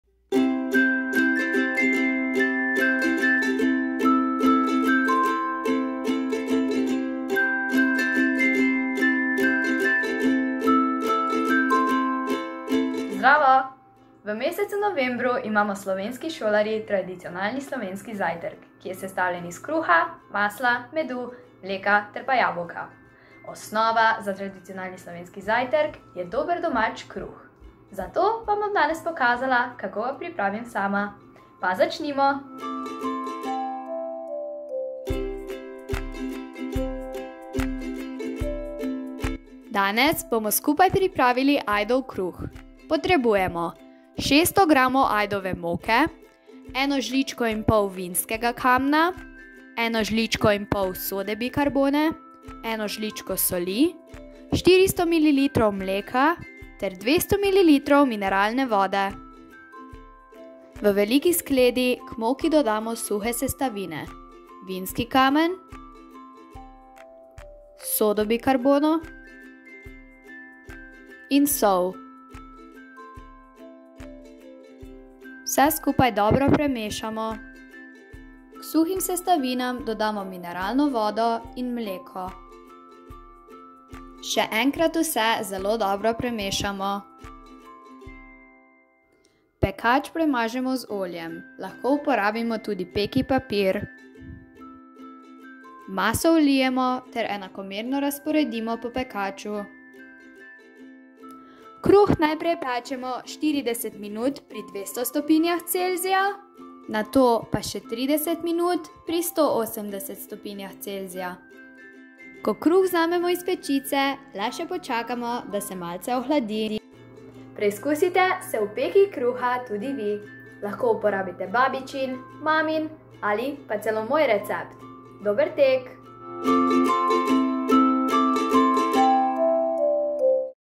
RTV Hruševec AJDOV KRUH
Ob Slovenskem tradicionalnem prazniku smo za vas pripravili prispevek, recept za ajdov kruh. Dober tek!